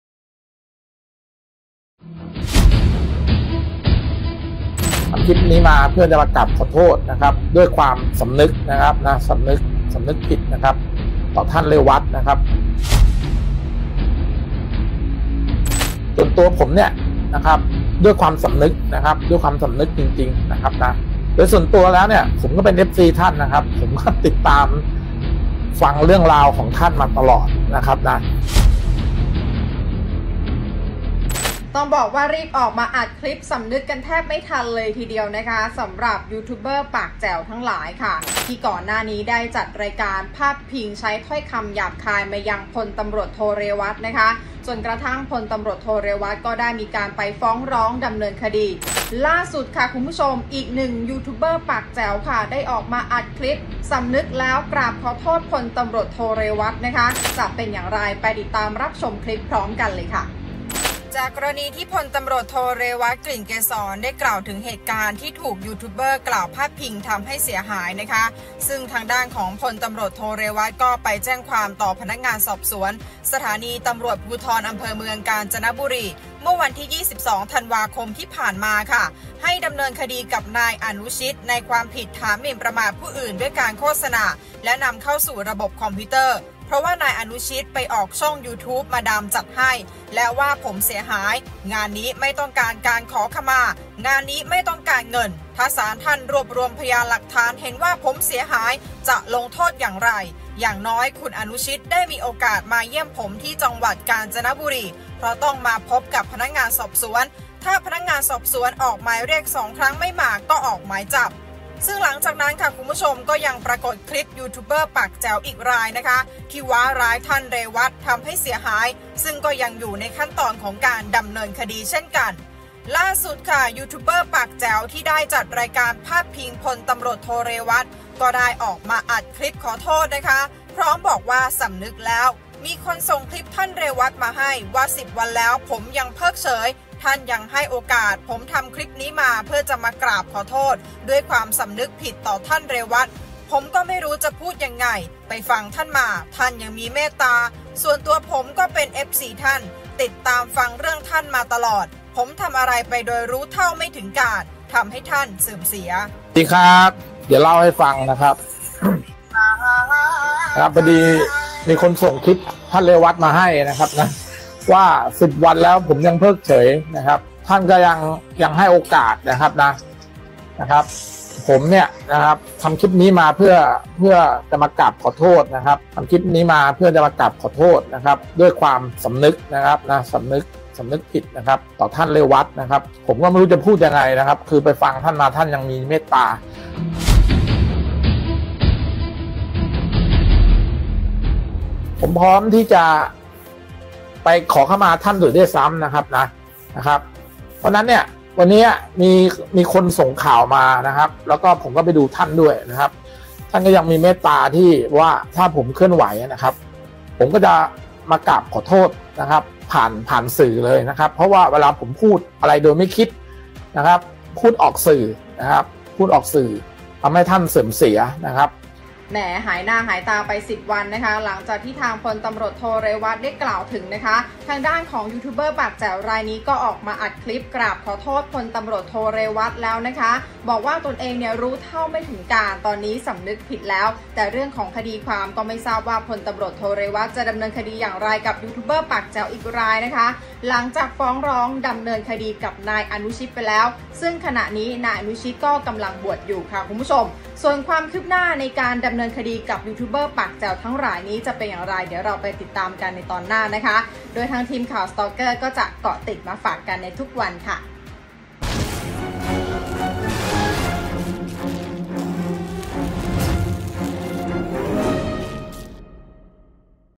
“ยูทูปเบอร์ปากแจ๋ว” ขอโทษ “พล.ต.ท.เรวัช” สำนึกผิดแล้ว จริงๆผมเป็น FC ท่าน | STALKER
“ยูทูปเบอร์ปากแจ๋ว” เสียงเศร้า อัดคลิปกราบขอโทษ “พล.ต.ท.เรวัช” สำนึกผิดแล้ว จริงๆผมเป็น FC ท่าน